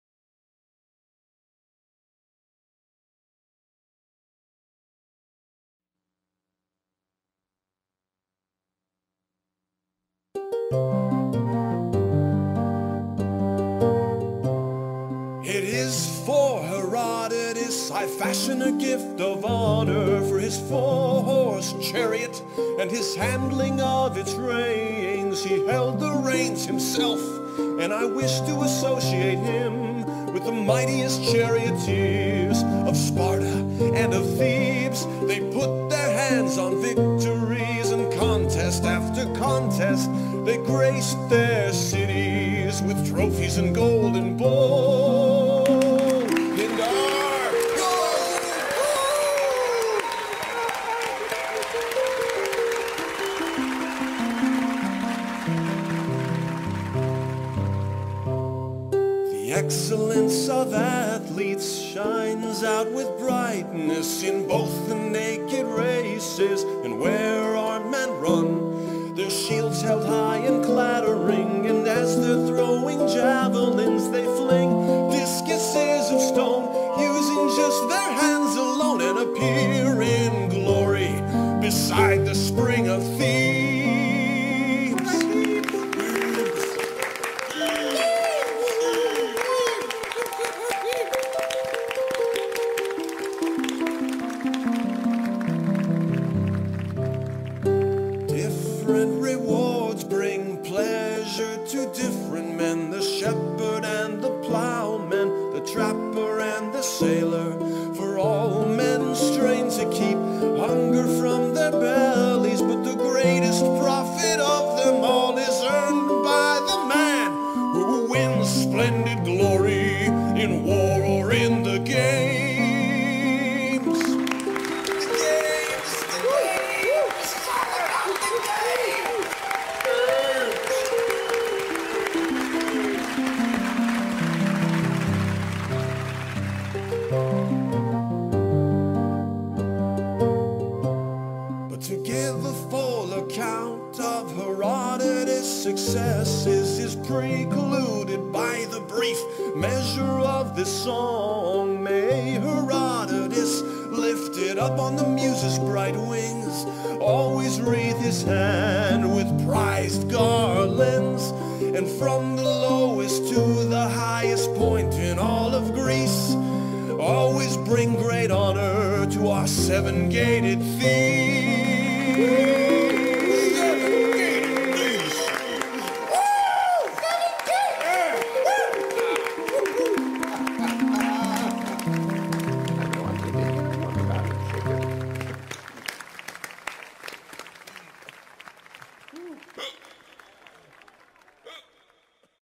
Pindar and the Charioteer
If the winner of the World Cup or Super Bowl commissioned a special song written on behalf of their victory, you'd have a situation much like that of the Ancient Greek poet Pindar, who wrote victory songs about athletic games. - the home of the Literature and History podcast.